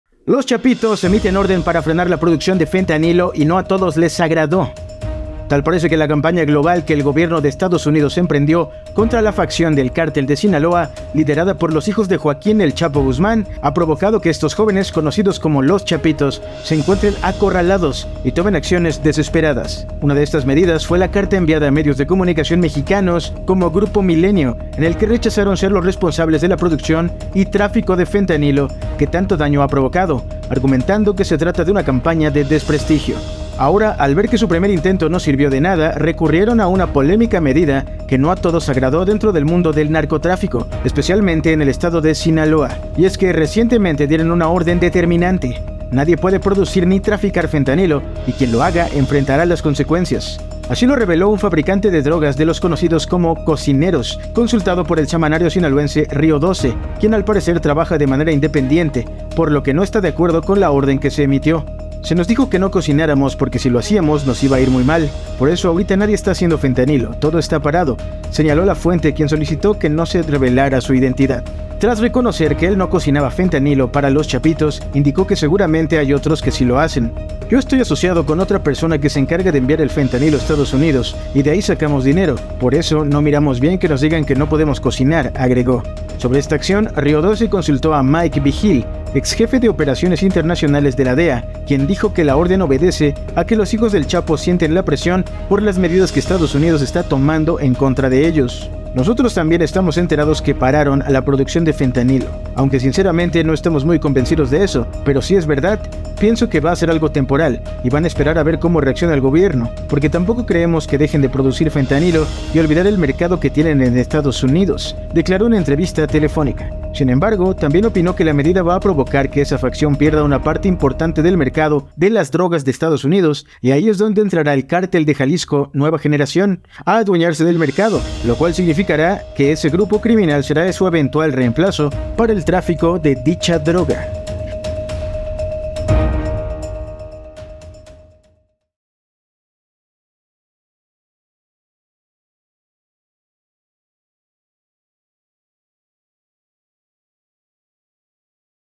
Los Chapitos emiten orden para frenar la producción de fentanilo | La Opinión
La campaña global del gobierno de Estados Unidos contra Los Chapitos, una facción del Cartel de Sinaloa liderada por los hijos de "El Chapo" Guzmán, ha acorralado a estos jóvenes, llevándolos a tomar medidas desesperadas, como ordenar que nadie produzca ni trafique fentanilo, y quien lo haga enfrentará las consecuencias. El exjefe de operaciones internacionales de la DEA, Mike Vigil, opina que esta medida provocará que Los Chapitos pierdan una parte importante del mercado de drogas en Estados Unidos.